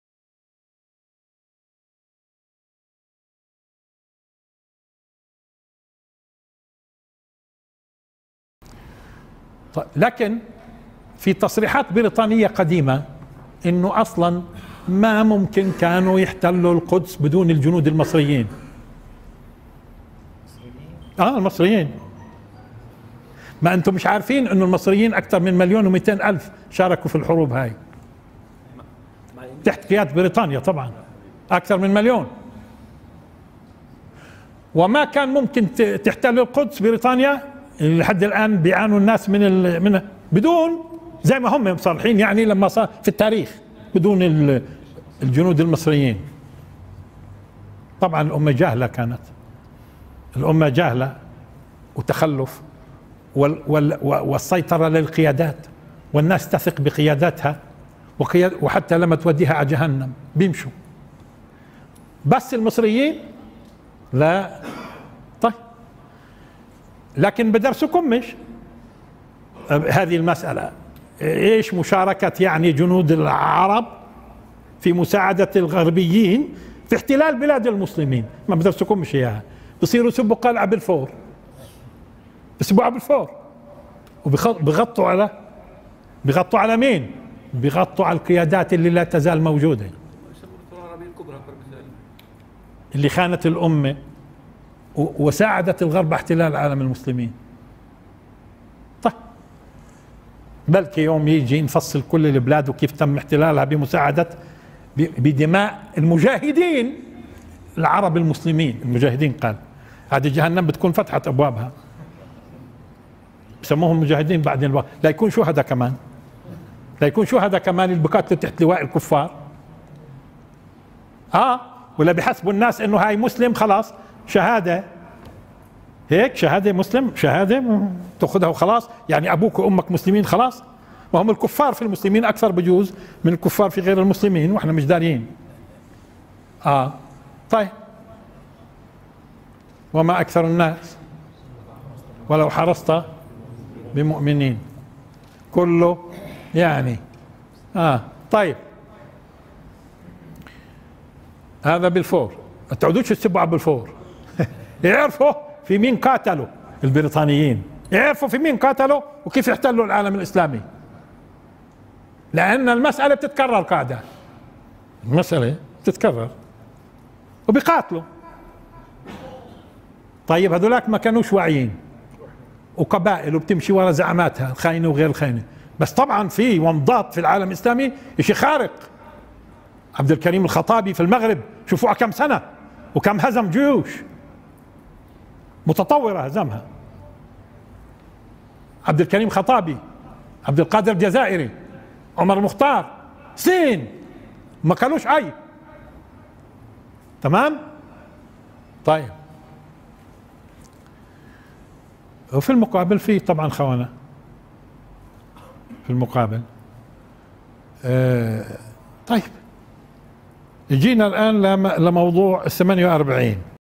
اخطر ما قاله الشيخ_بسام_جرار دور المصريين فى احتلال بريطانيا القدس 1917 م
دروس_الشيخ بسام